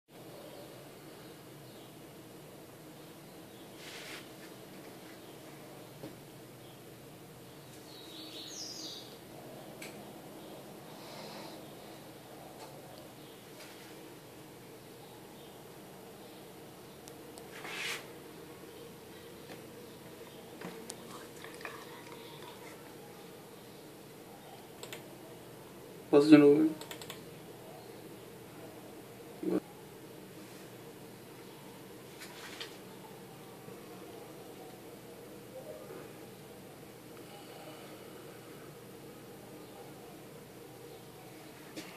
Câmeras flagram assalto no Feira X
Assalto aconteceu por volta das 9h30.